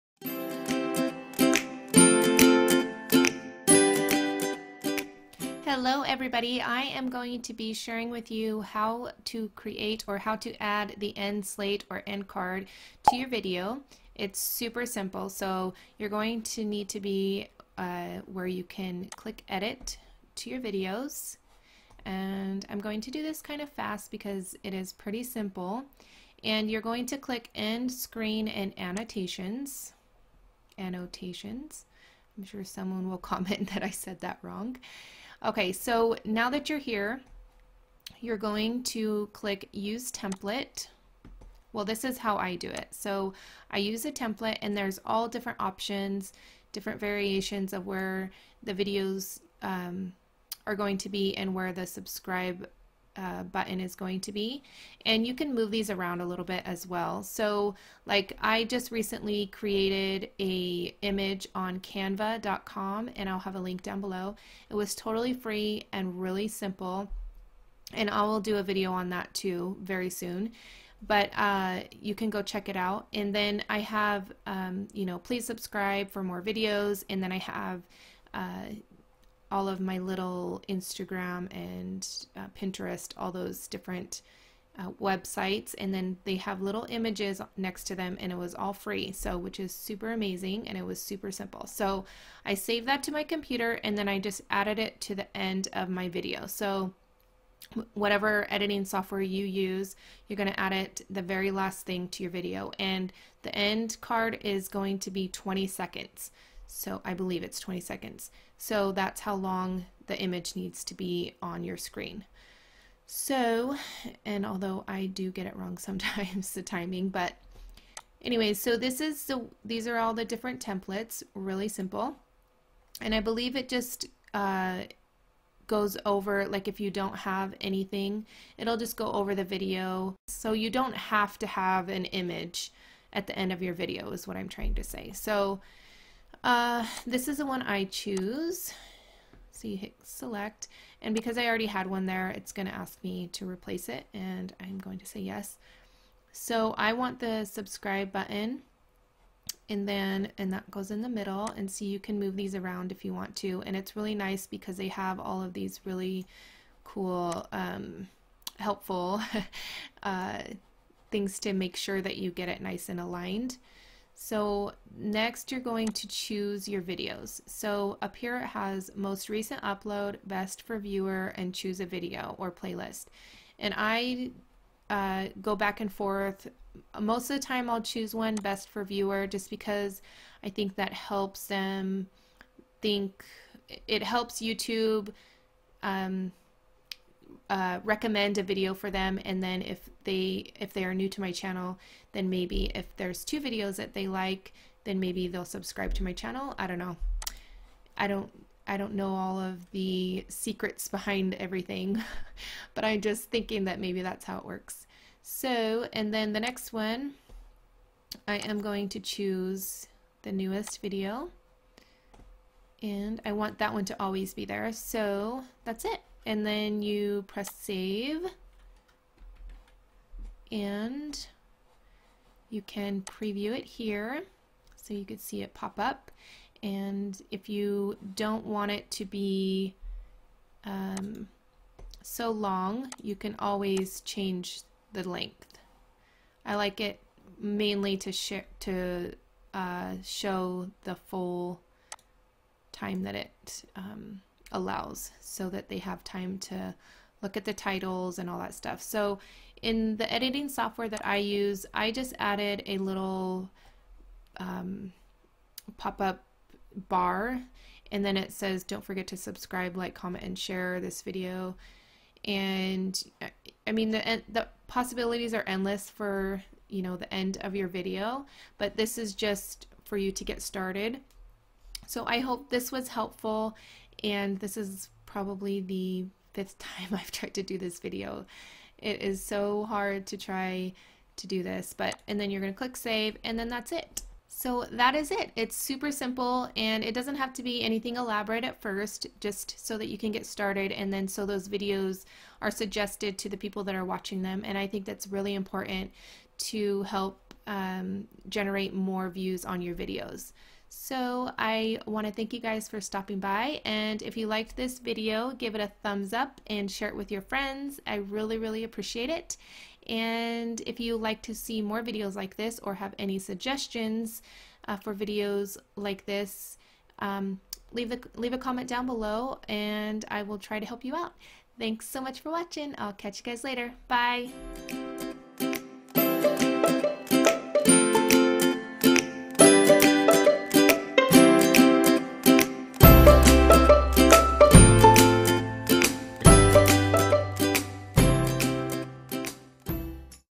How to add youtube end cards to your videos
I share how to add the youtube end card to my videos.

Camera I use
Video editing software - Filmora